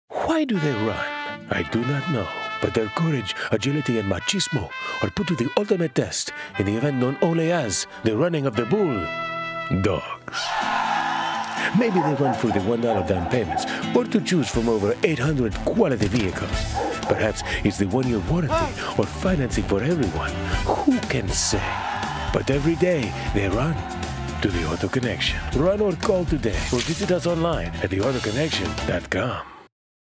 Running of the . . .
This is the version of the commercial which debuted on February 3, 2013, during Super Bowl 47. Using 15 bulldog actors and 15 human actors, we shot this commercial in a day. We paid to have a street shutdown in Norfolk, Virginia so we could replicate Pamplona, Spain. Be sure to watched the Extended Director's Cut, too. More at TheAutoConnection.com